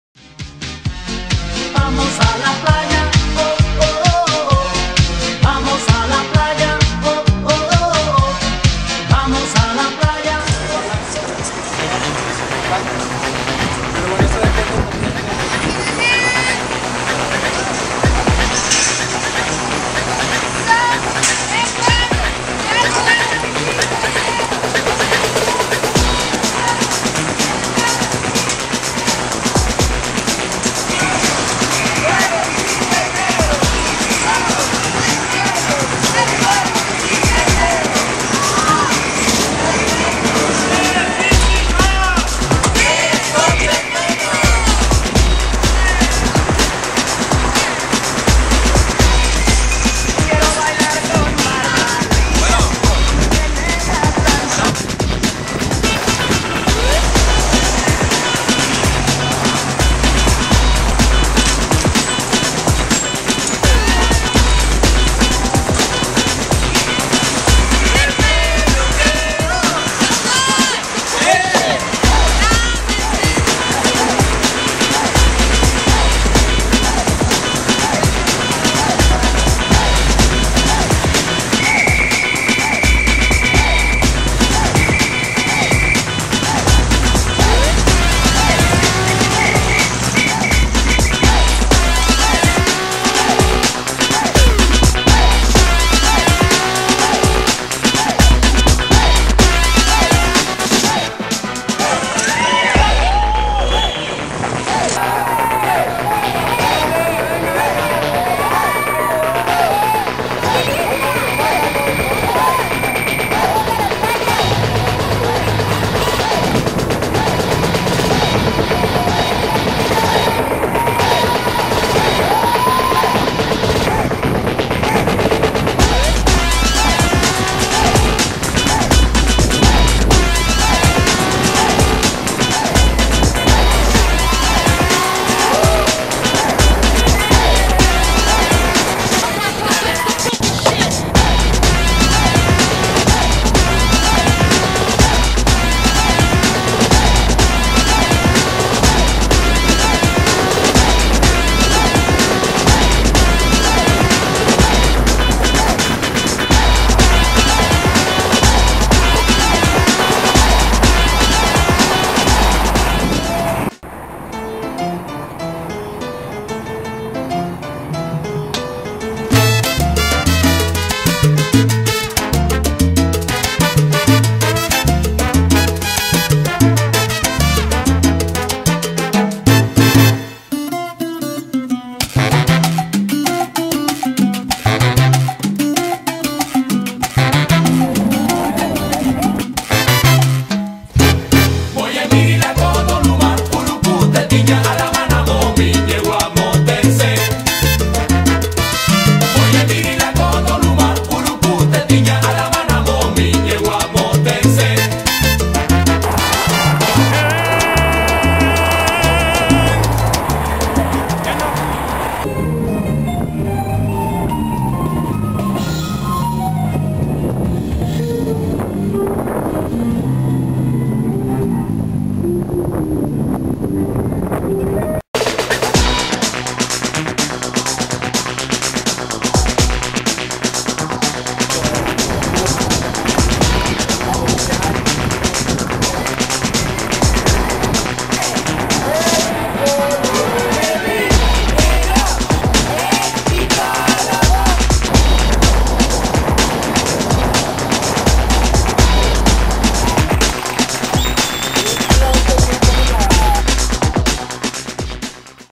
Al agua bici | Pueblo Bicicletero
|| Este domingo 13 de junio fue de albercazo en el Parque España. De Colegio Civil salimos a recorrer parte del centro de la Ciudad por avenidas como Juárez, Madero, Zaragoza, Aramberri; cruzamos el Parque Fundidora para llegar al Parque España, donde tuvimos la oportunidad de refrescarnos con la sombra de enormes árboles y una alberca gigante.

La ruta, los parques, el albercazo y, sobre todo, la buena vibra de la banda bicicletera hicieron de este un recorrido para el recuerdo.